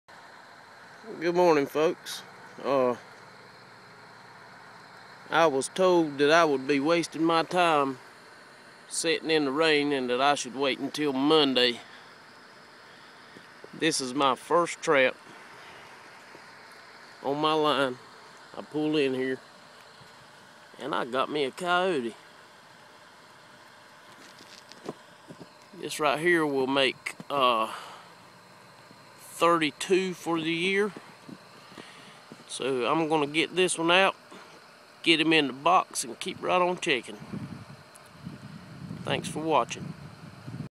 Rainy day yote!
This coyote is #32 for the season. Every off day I've had lately has been sloppy wet. This week was no exception, however, I'm suffering through it with some reward. The moon is also full, so its a do